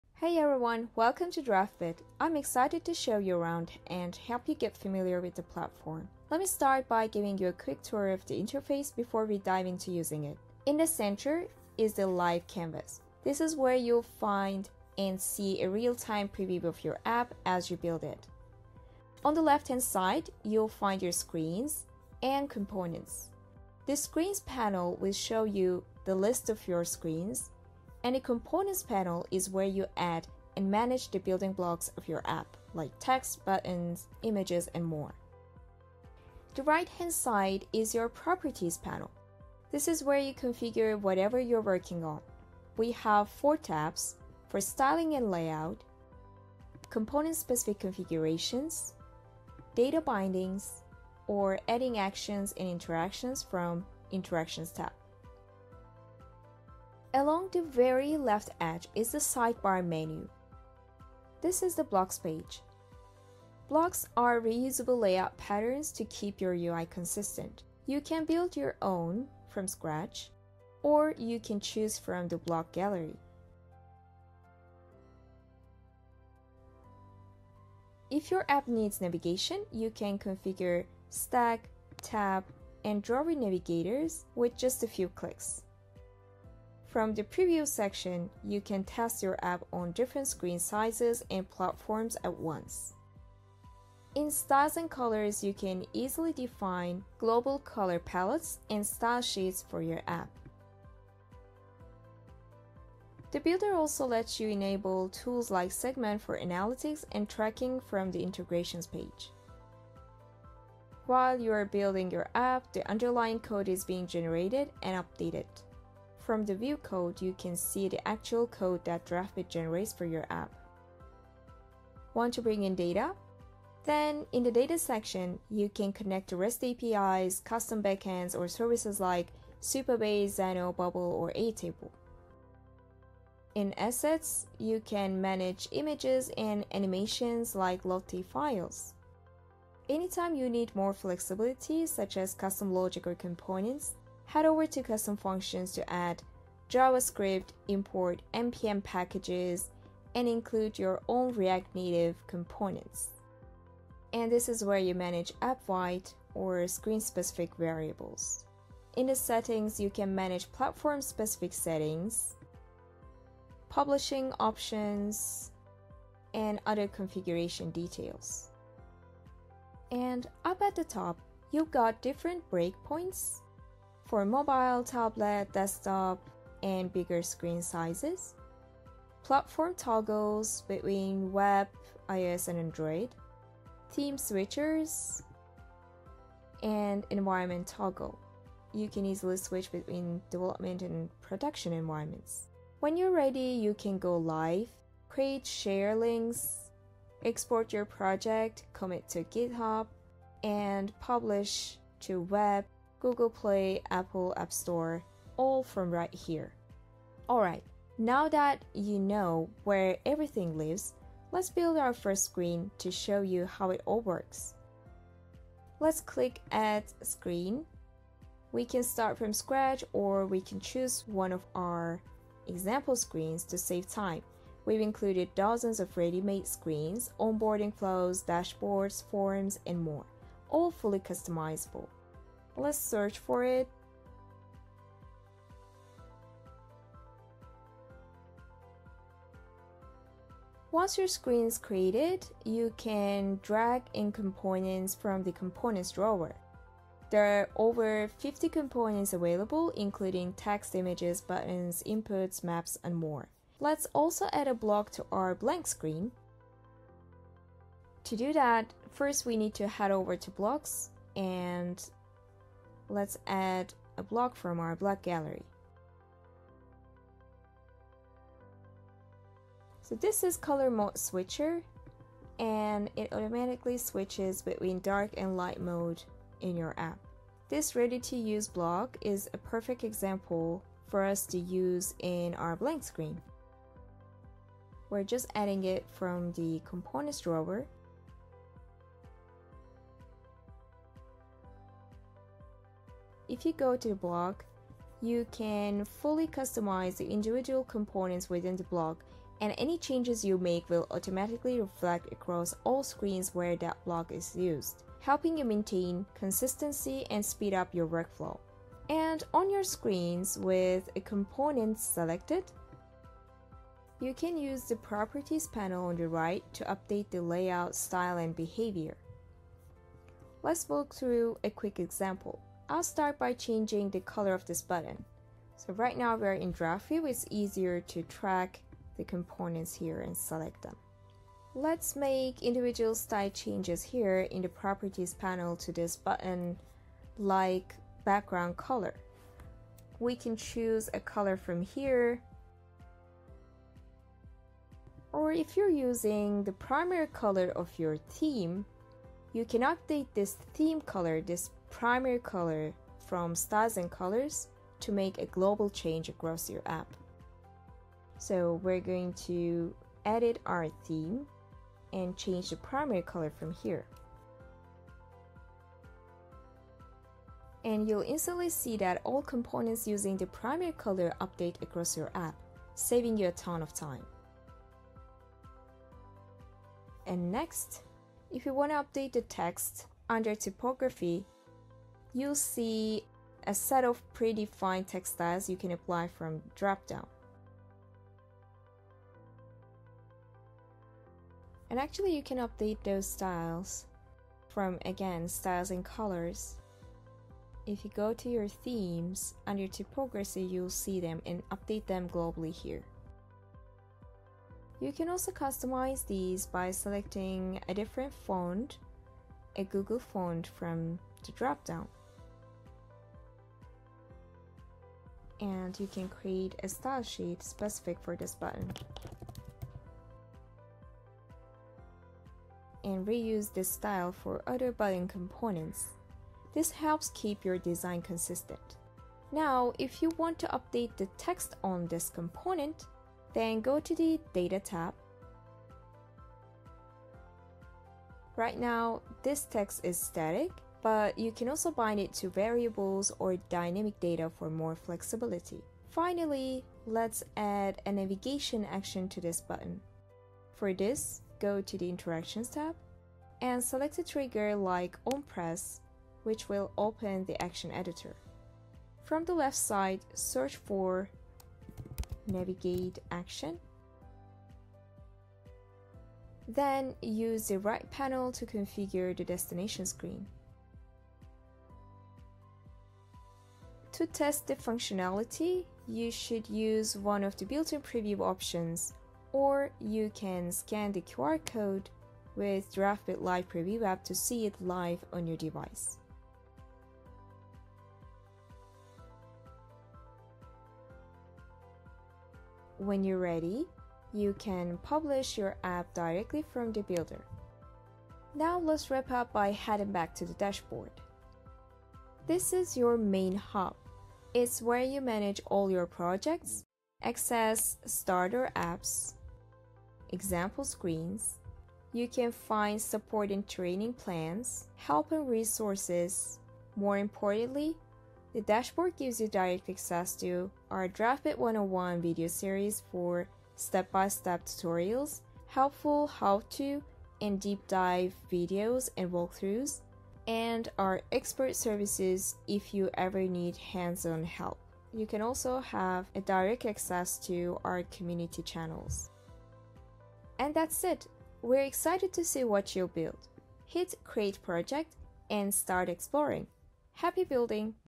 Intro to Draftbit
Want to build apps for the web, iOS, and Android with or without coding? Draftbit is a premium visual app builder that's easy enough for no-coders, and powerful enough for developers.

Get started in 5 min! Try Draftbit for free